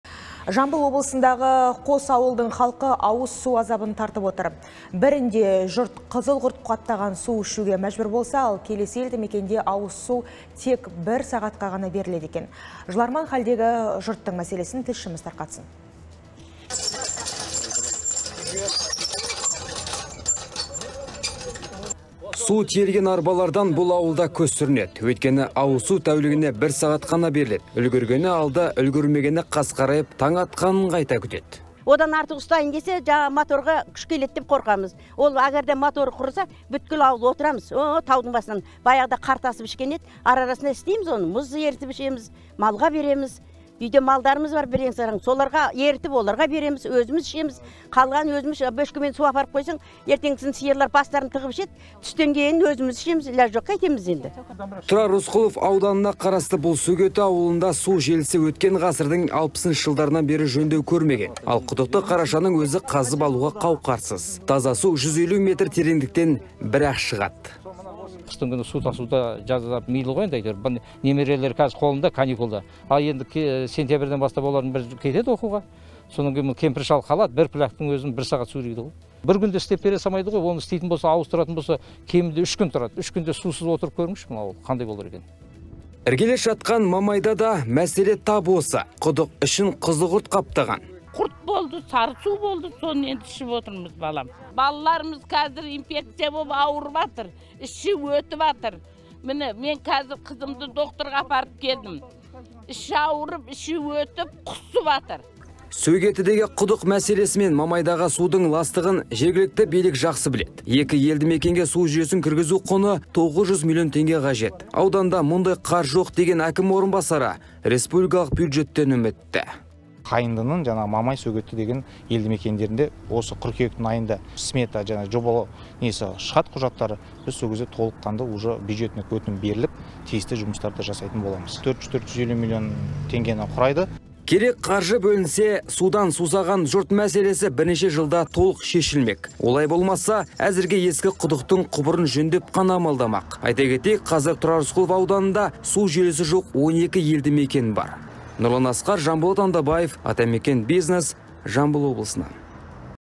Ауыл жұрты қызыл құрт қаптаған суды тұтынып отыр
Жамбыл облысындағы қос ауылдың халқы ауыз су азабын тартып отыр. Бірінде жұрт қызыл құрт қаптаған су ішуге мәжбүр. Ал келесі елді мекенде ауызсу тек бір сағатқа беріледі. Жыларман хәлдегі жұрт мәселесін тілшіміз тарқатады.